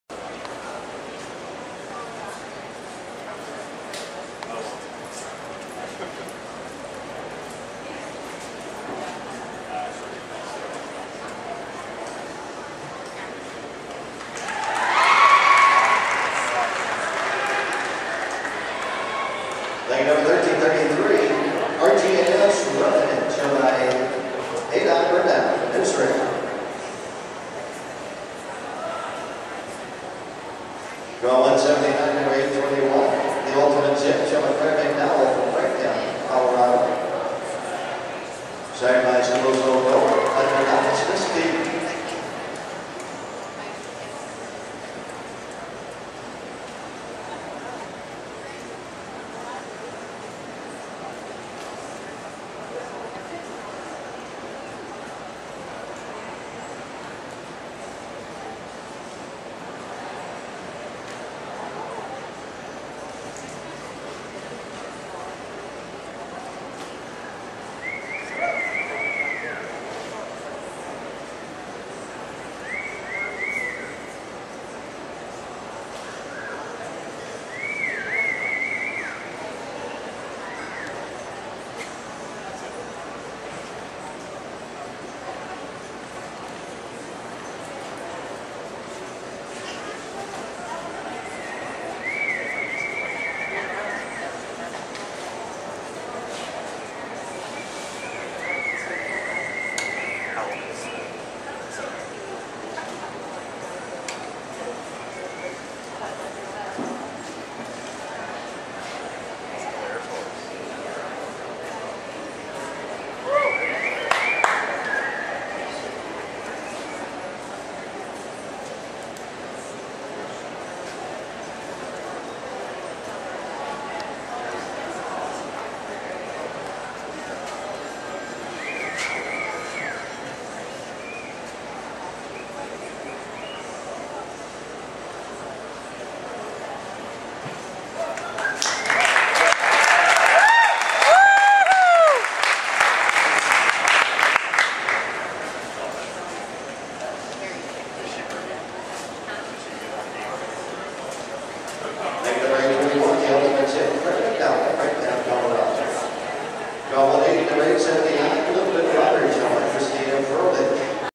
Claire McDowall Horsemanship Prelims 2014
Claire McDowall competing in the Horsemanship pre-lim event at the 2014 AQHYA World Show.